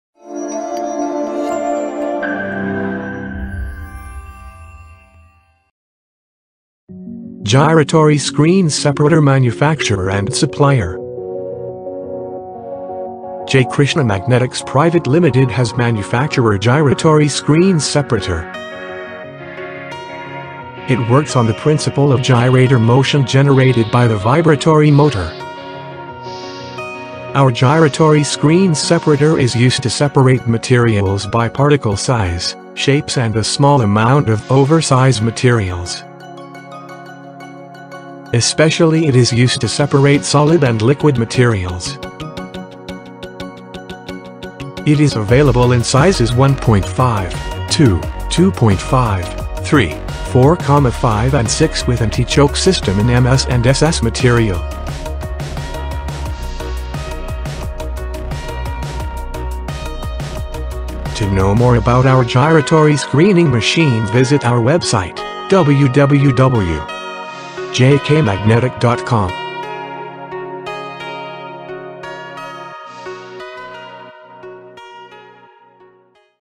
Gyratory Screen Separator - Manufacturer & Supplier
Jaykrishna Magnetics Pvt. Ltd. is India's leading manufacturer and supplier of Gyratory Screen Separator which is widely used in the industrial equipment for separation, sizing and grading.

Our circular/gyratory screen separators are the most advanced vibratory screening machine available in the market. Our machine is accurate because of its grading functionality of different sized particles.

To know more about our gyratory screening separator, visit our